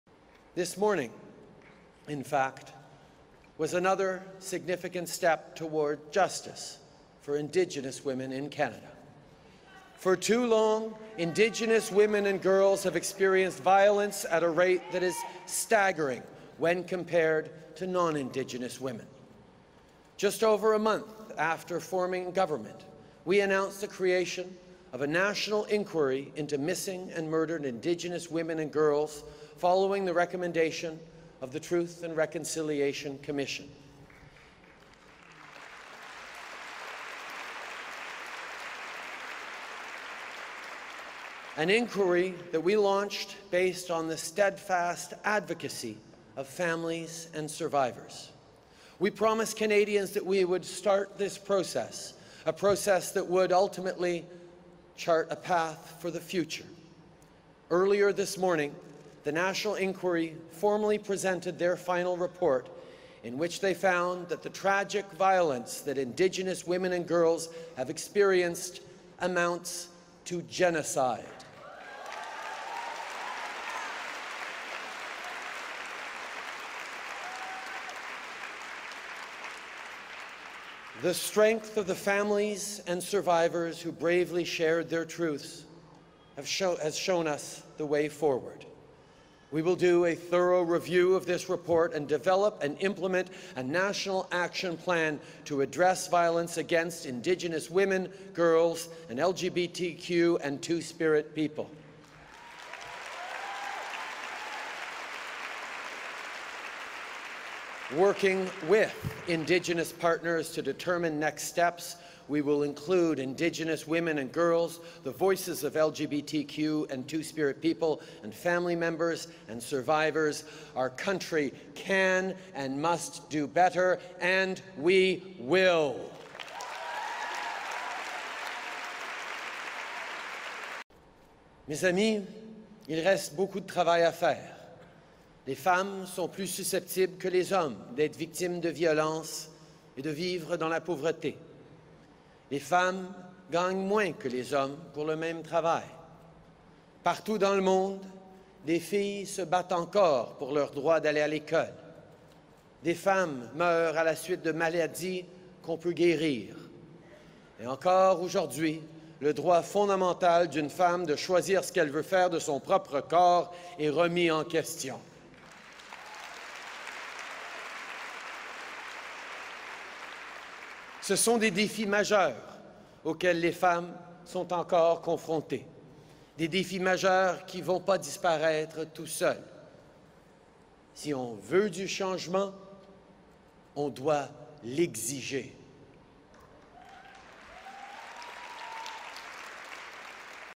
Trudeau calls MMIWG report 'genocide,' speaks out subtly against new abortion laws
Earlier on Monday, Prime Minister Justin Trudeau promised that his government would fail Indigenous women and girls “no longer,” as the report of the National Inquiry into Missing and Murdered Indigenous Women and Girls (MMIWG) was released.

“Earlier this morning, the national inquiry formally presented their final report, in which they found that the tragic violence that Indigenous women and girls have experienced amounts to genocide,” he said.

He had refrained from using the word "genocide" when pressed by an audience member earlier in the day.

Trudeau also remarked that women's rights to make choices with their bodies are under attack.

The event was a conference aimed at policy changes “for the health, rights, and wellbeing of girls and women and achieving a more equal world.”